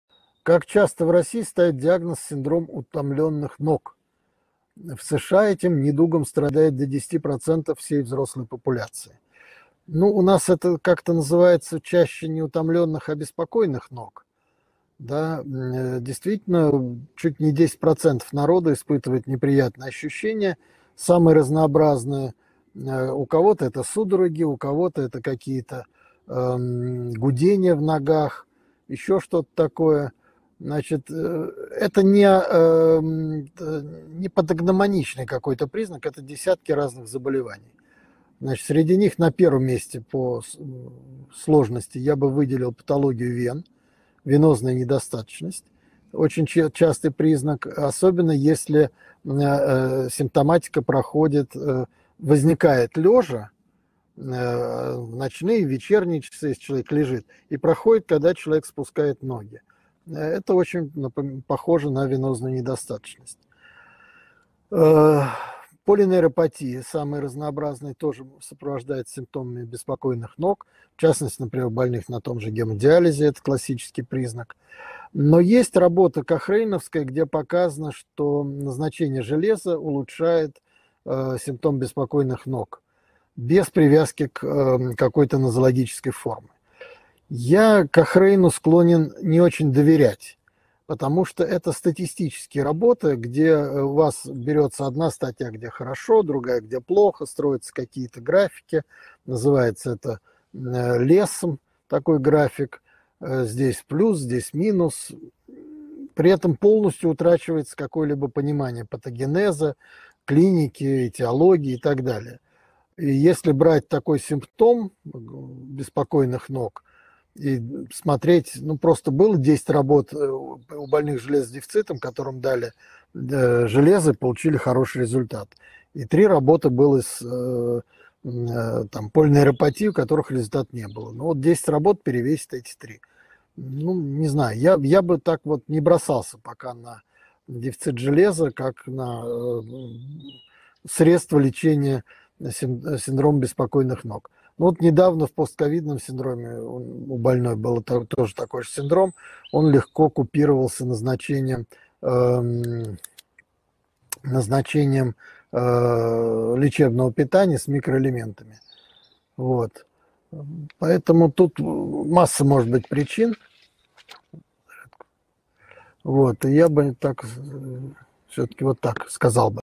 Часто в России ставят диагноз – синдром утомленных ног? В США этим недугом страдает до 10% популяции
Ответы на вопросы о дефиците железа подготовлены в рамках Высшей Школы Терапии Москвоского Городского Научного Общества Терапевтов.
На вопросы отвечают:
Председатель Правления МГНОТ профессор Воробьев Павел Андреевич
к.м.н., доцент кафедры общей врачебной практики Первого МГМУ им. И.М.Сеченова, Краснова Любовь Сергеевна

Записаться на консультацию по вопросам железодефицитных состояний можно на сайте Медицинского Бюро

Хотите посещать наши мероприятия и вебинары?

и в Instagram чтоб быть в курсе наших активностей.